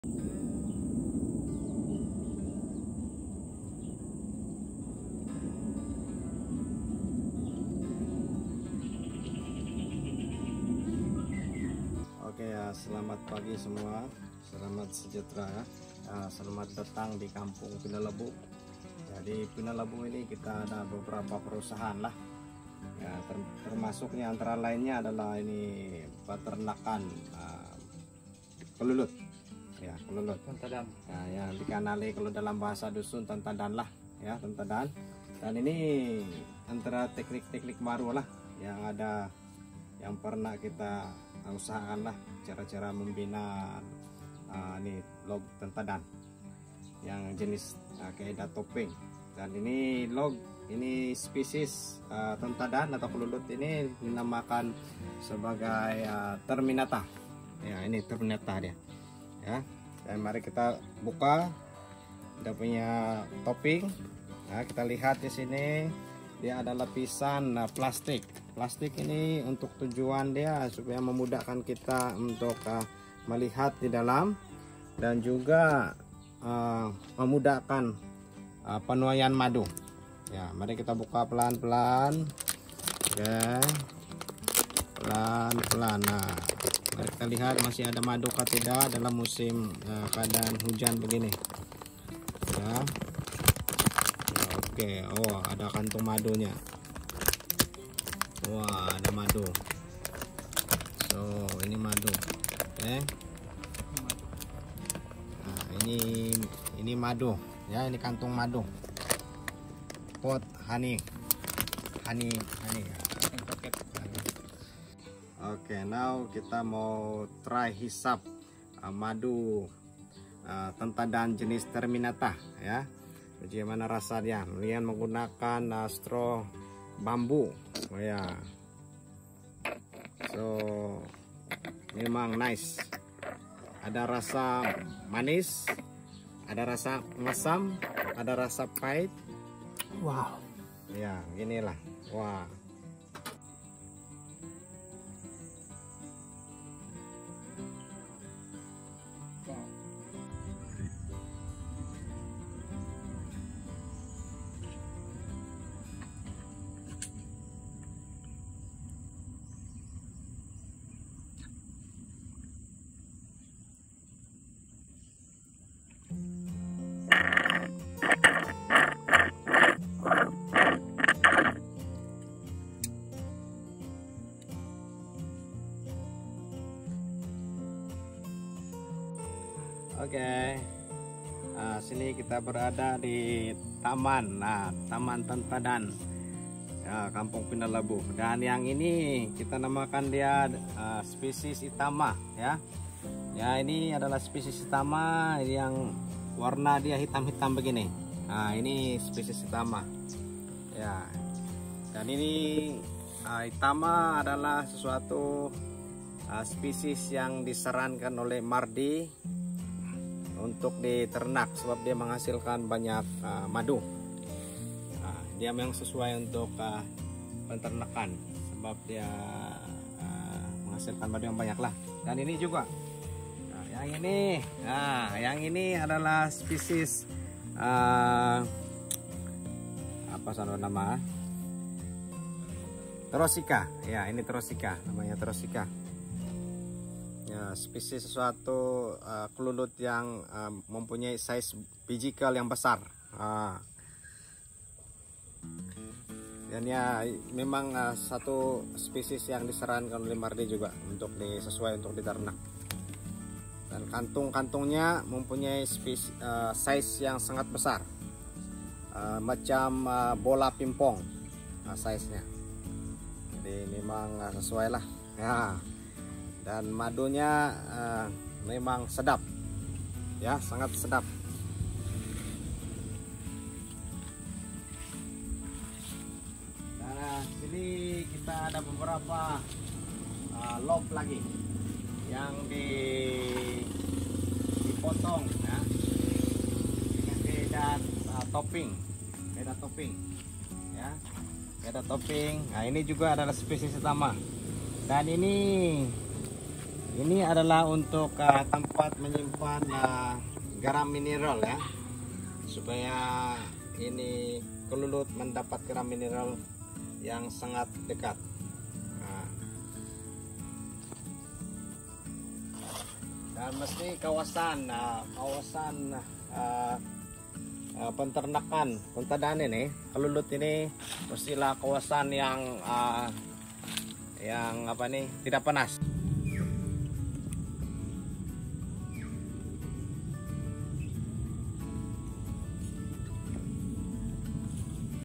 Kelulut (Stingless Bee) Beekeeping in Sabah (English subtitles available)
Kelulut (Stingless Bees) beekeeping is a growing industry in countryside of Sabah, Malaysia. Closely related to honeybees and known as Tantadan in local Dusun language, stingless bee produces honey which is rich in anti-oxidant and contains nutrients that improve immune system. Stingless bee honey is mainly sweet, usually with a mix of sour or bitter taste. Though the quantity of stingless bee honey is small, it's in great demand and highly priced (about MYR20 every 100 Gram). Village Development Cooperative (KPD) also provides stingless bee farming course to help farmers to generate side income.

In this video is my tour to the stingless bee farm in Pinolobu Village of Kota Belud. They have about 70 bee houses in the farm and rear over 6 species of Kelulut. Stingless bees usually nest in cavity and trunk hollows. The villagers collect the stingless bee nest from the wild and move it to a log (called gelodok) with a roof covering the opening on top. The beekeepers can check out the honey production without destroying the bee hive, to ensure a sustainable harvest. During my visit, they let me try sipping the honey from the honey storage pot of the nest. The taste is sweet and sour with a bit of fruit flavour.